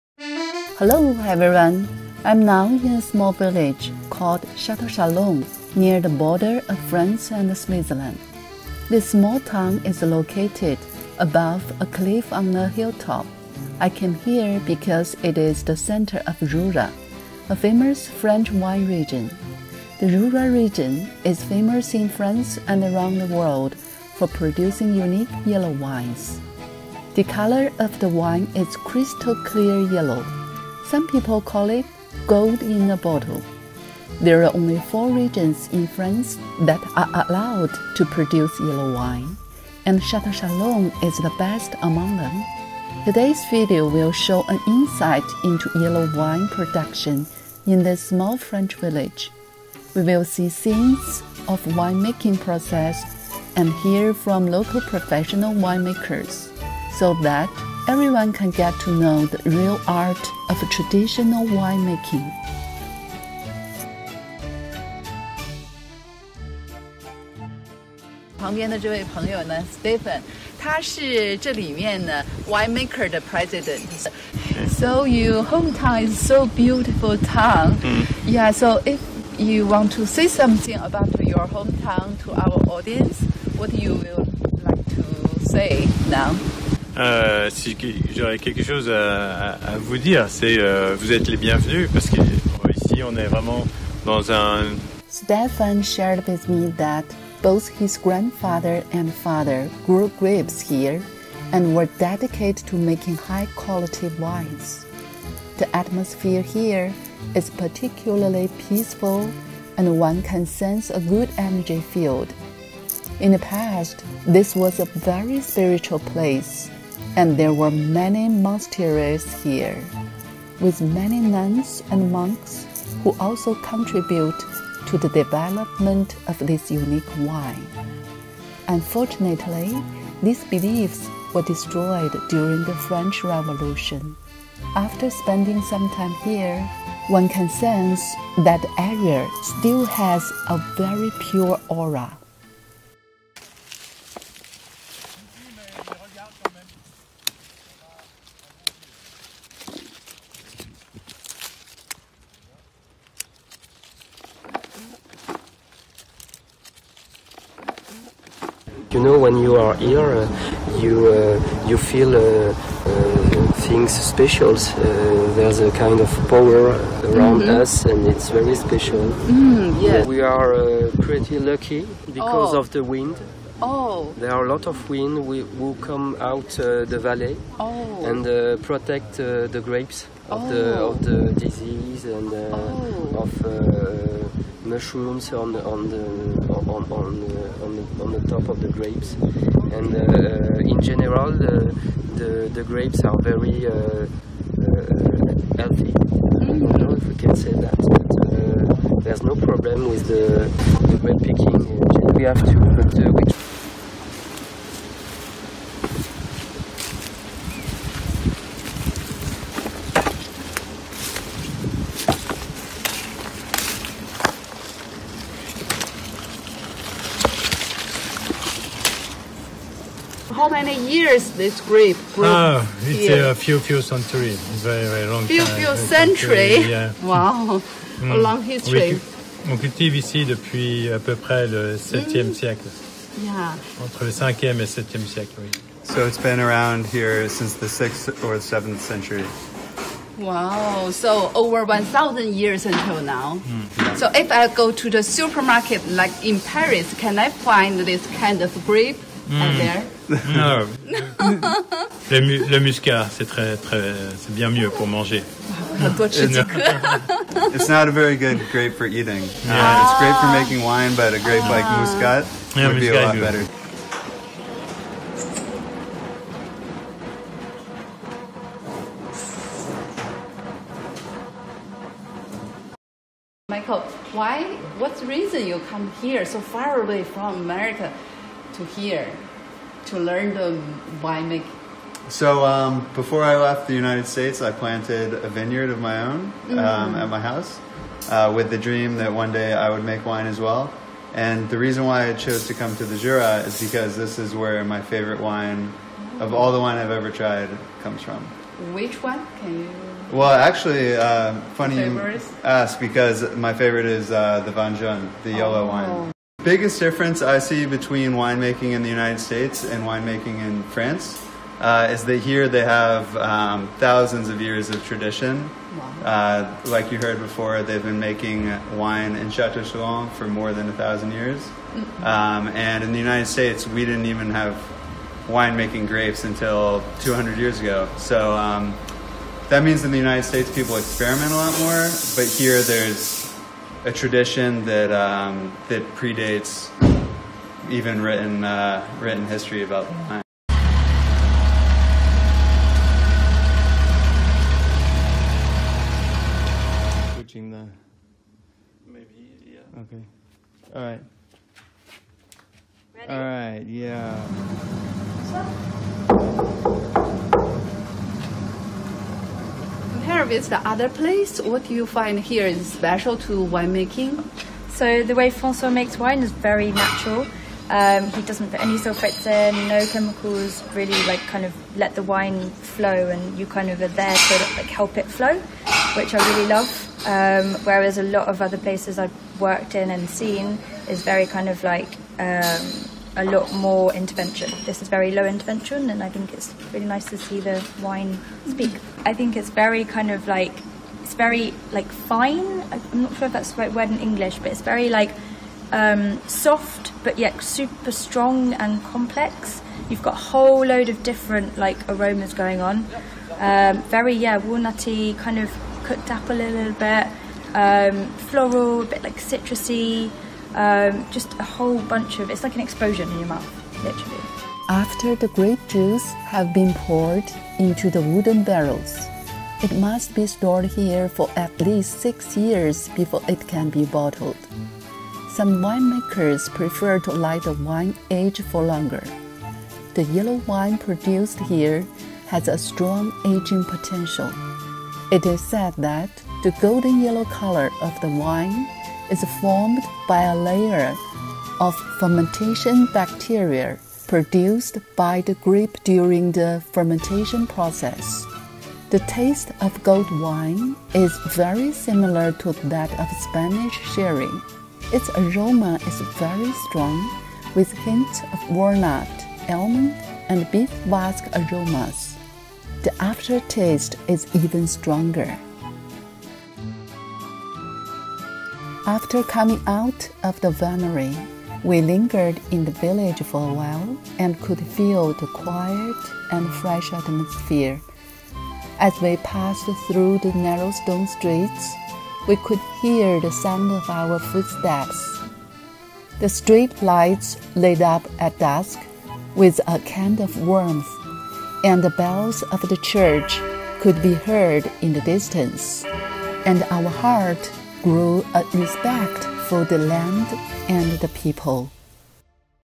France Vlog -- How Yellow Wine Is Making In Chateau Chalon
this video will show an insight into yellow wine production in this small French village. We will see scenes of the wine making process and hear from local professional wine makers so that everyone can get to know the real art of traditional wine making.

# France travel# yellow wine making#wine making in france